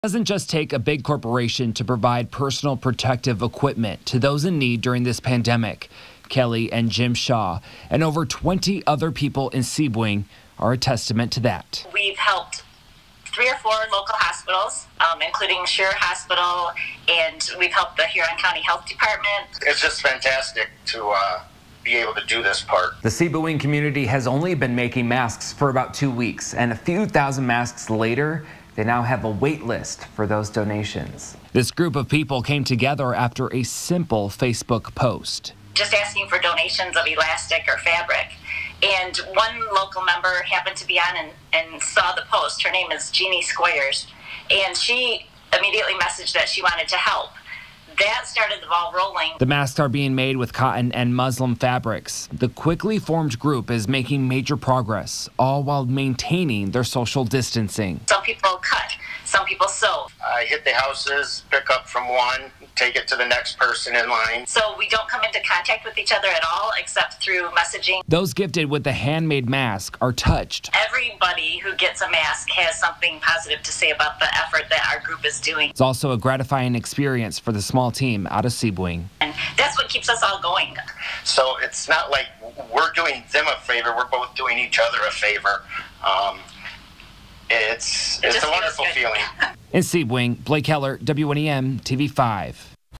Mid-Michigan group making masks for healthcare workers on frontlines
During the pandemic, we’ve seen multiple corporations provide or make personal protective equipment for healthcare workers on the frontlines.